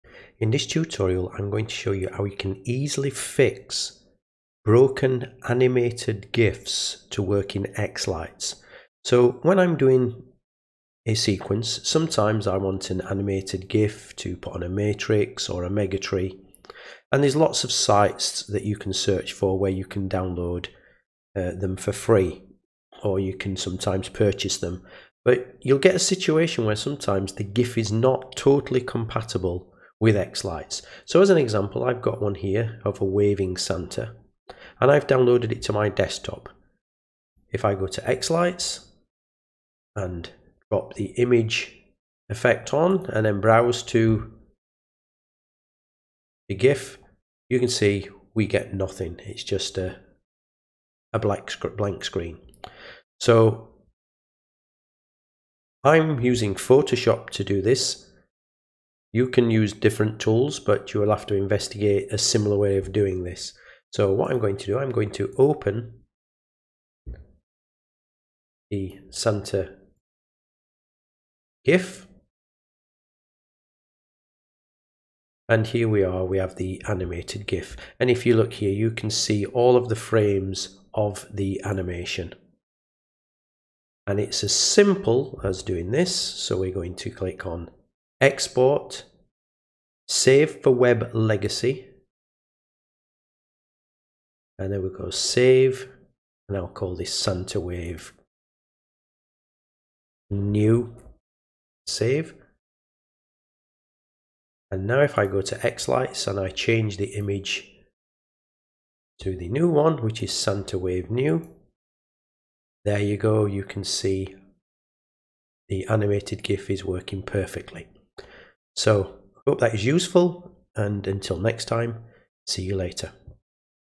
xLights - How to fix broken Animated GIFs
In this tutorial I show you how you can fix downloaded animated GIFs that are not compatible with xLights.